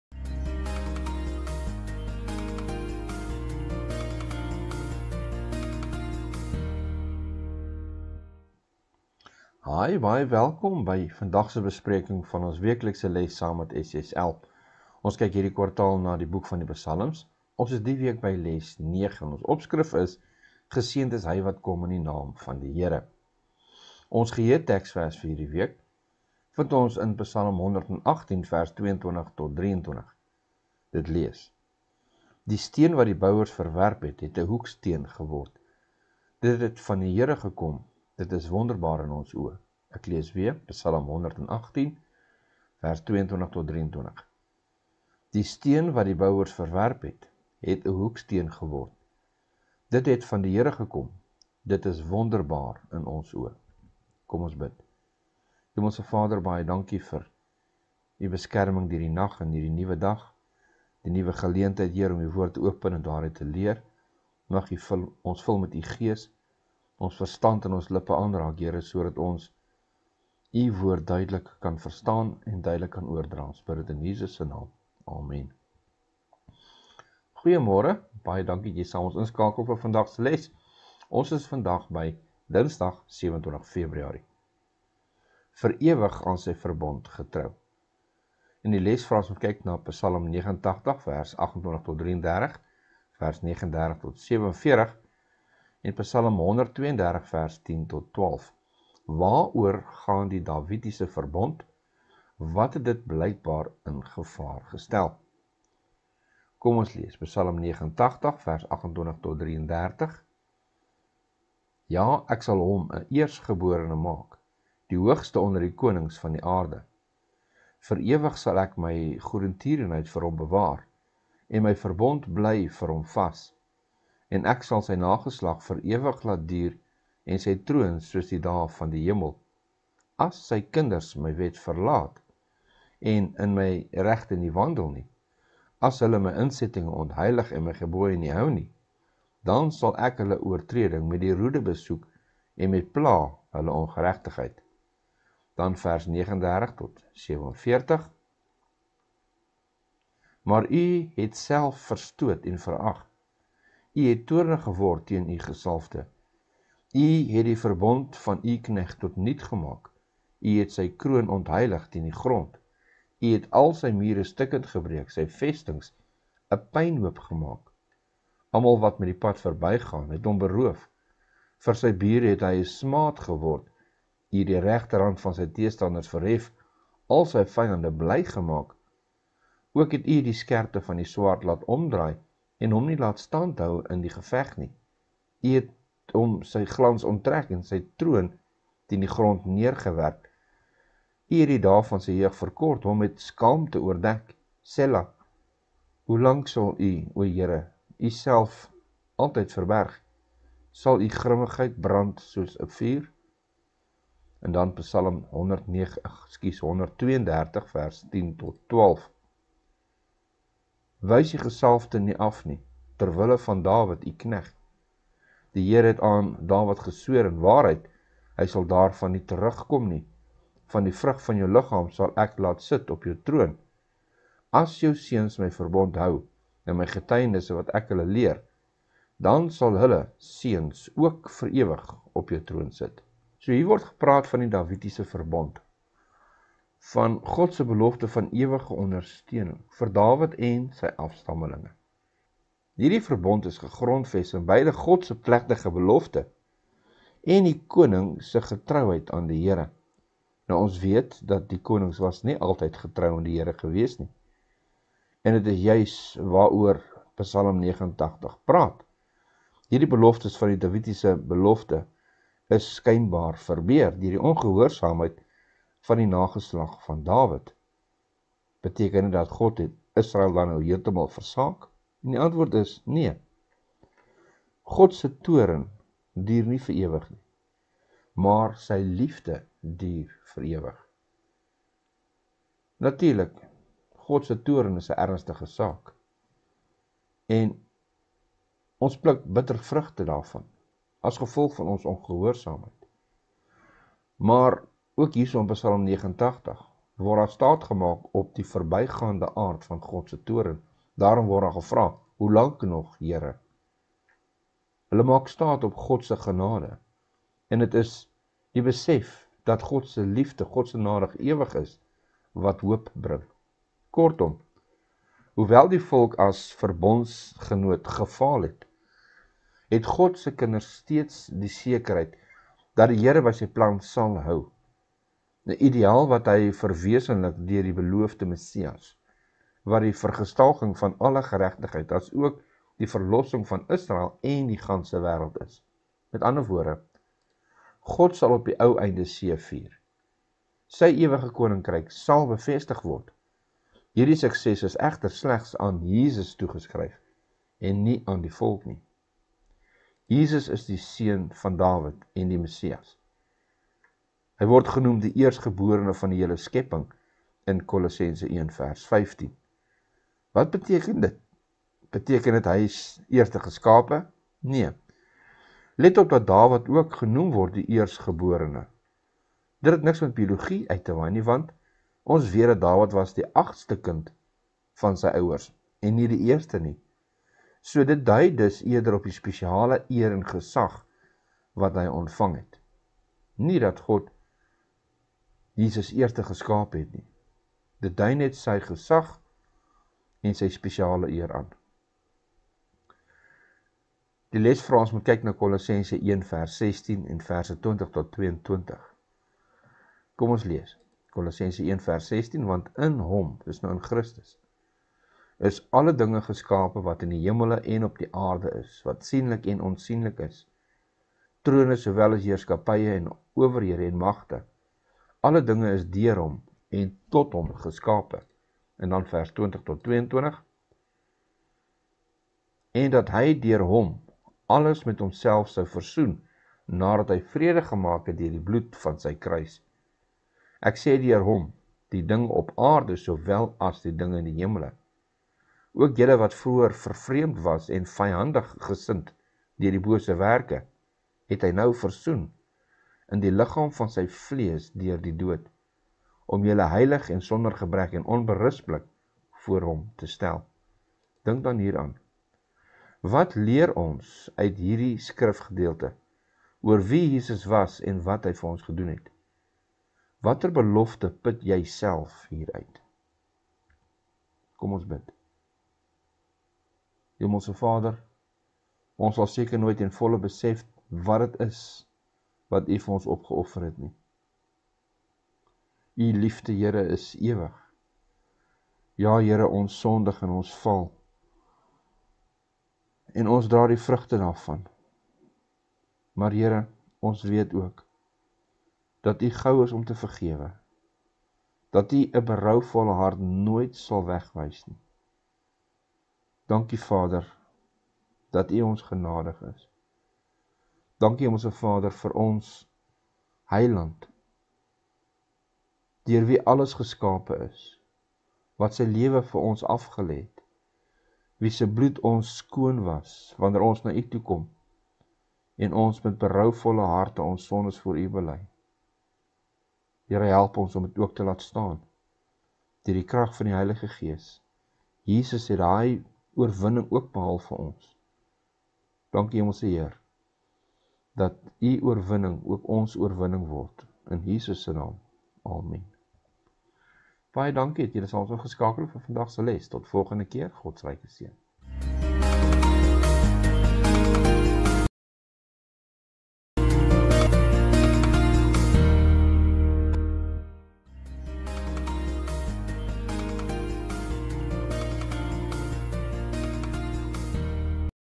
Dinsdag 27 Februarie - Vir ewig aan Sy verbond getrou.
Les 9 – Geseend is Hy wat kom in die Naam van die Here.
24 Februarie tot 1 Maart

Aanbieder:
David Kühn

CREDITS:
All these videos and music are from YOUTUBE under

Jesus Christus.